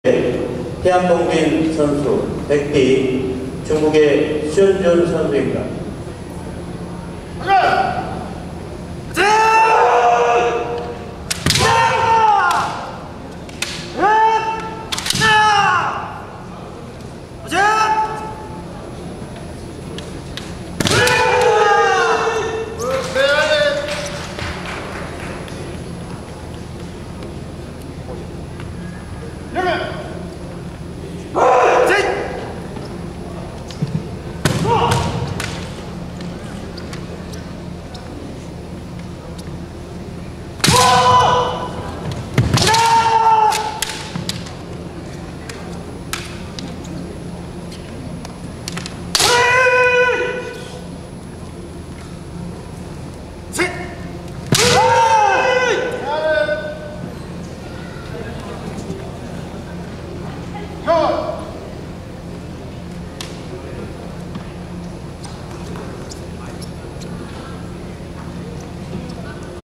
2016 청주세계무예마스터십 검도대회 M I 3dan R CHN2 Tian CHN EN4 Sun
2016 CheongJu World Martial Arts Masterships Kendo
| Man's Individual League
2016 청주세계무예마스터십 검도대회
| 3단이하 남자부 개인전 예선리그